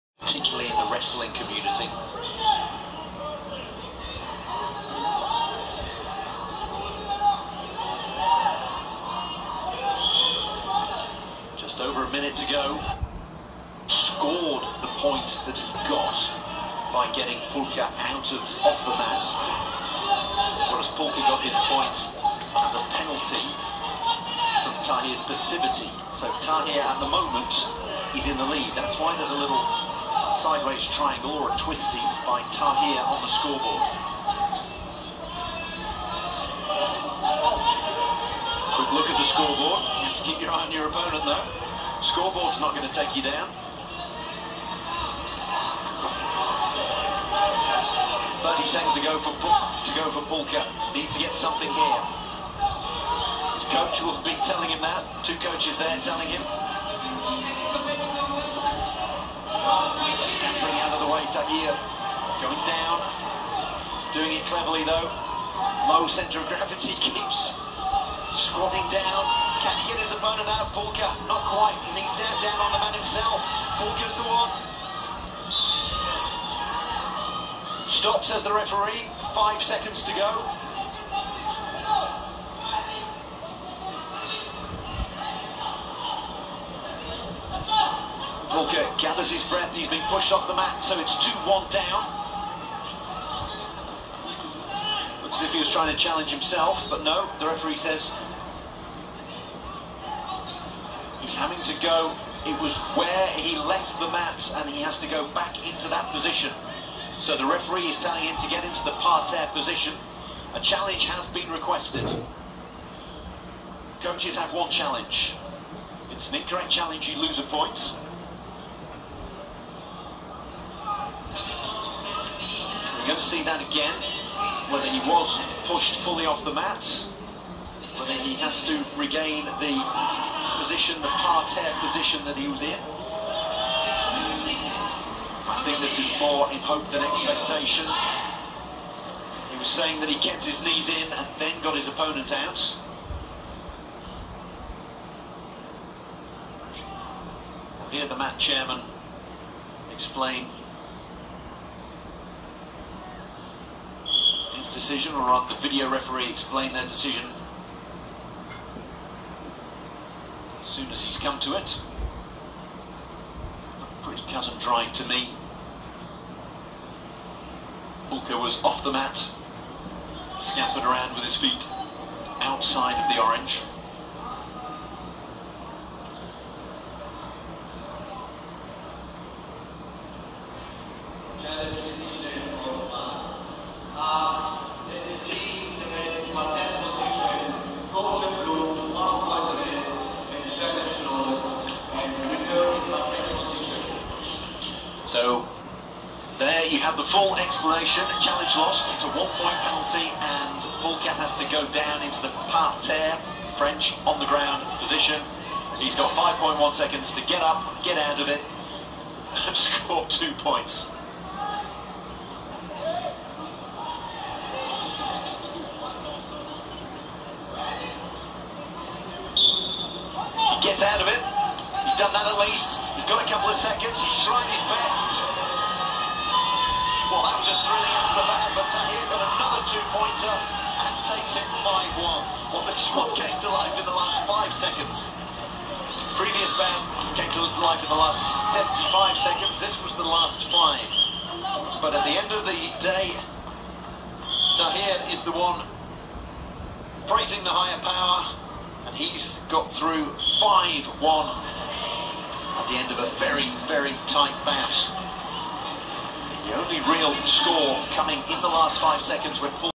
Mohammad Sharif Tahir Quarter Final winner Freestyle wrestling Common wealth games 2022
And